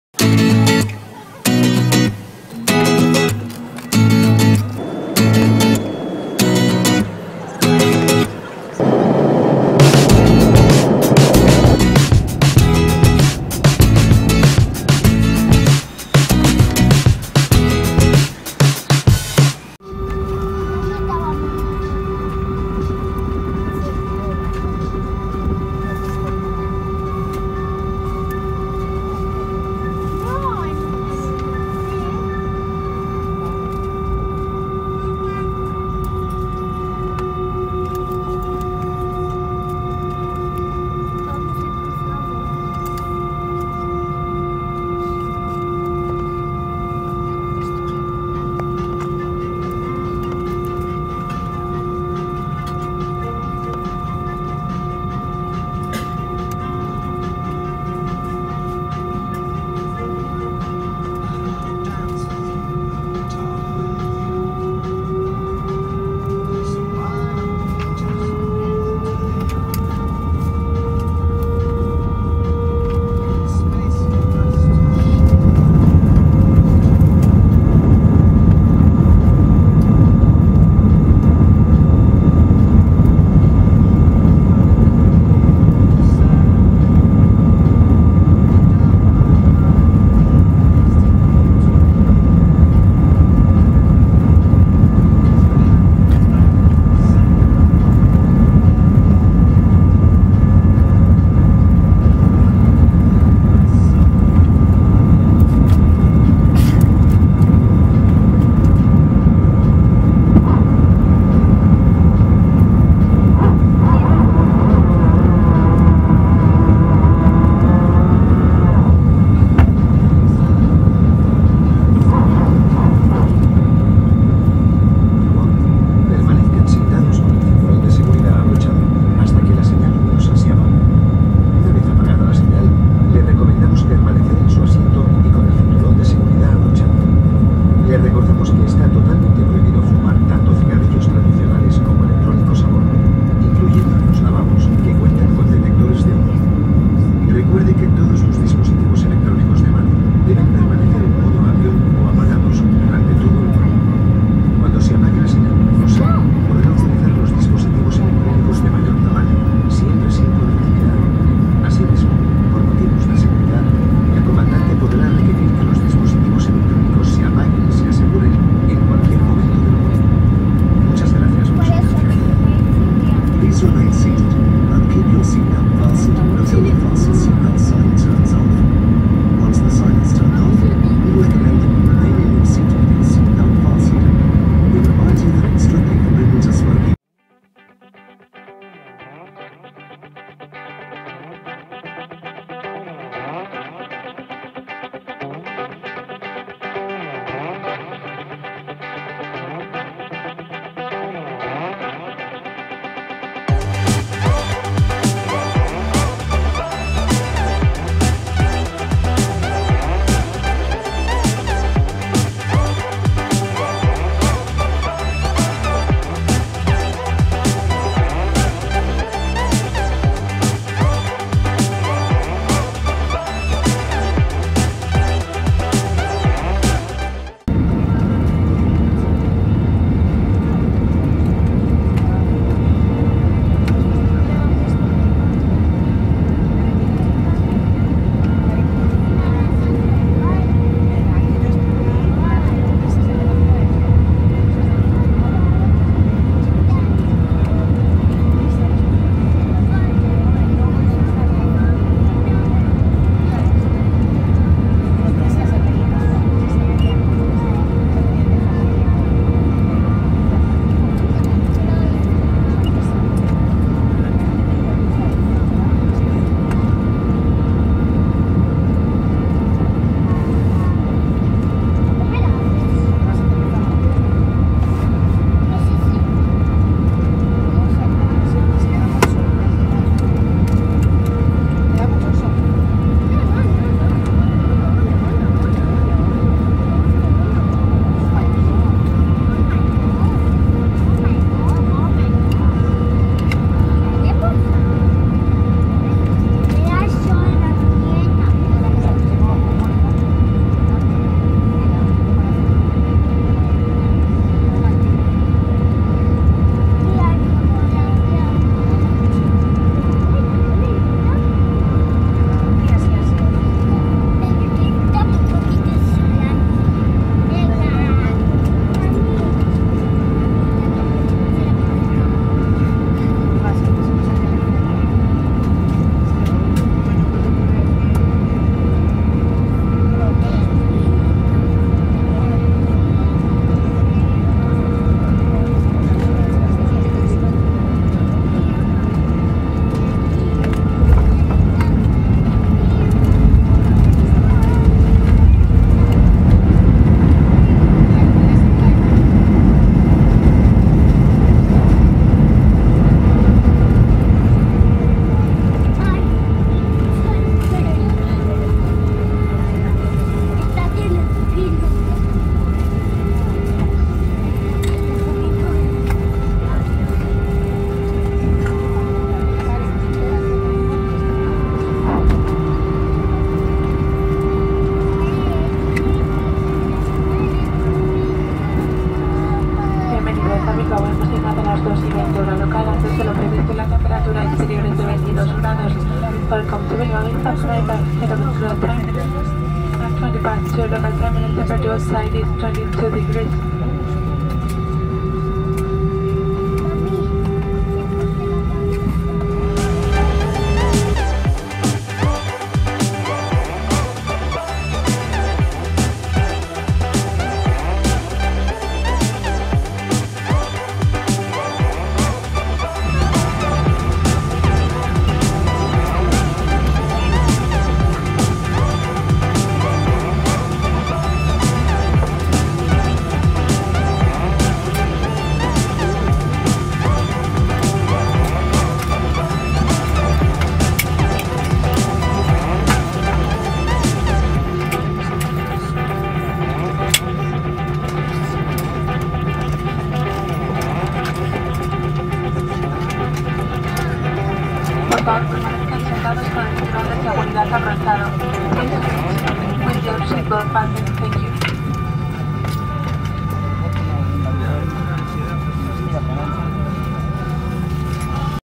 Vueling A320: A Coruña to Bilbao Take Off and Landing [FullHD]
Flight: VY1463 LECO - LEBB
Aircraft: Airbus A320-232(WL)
Reg: EC-LVS
Seat: 17A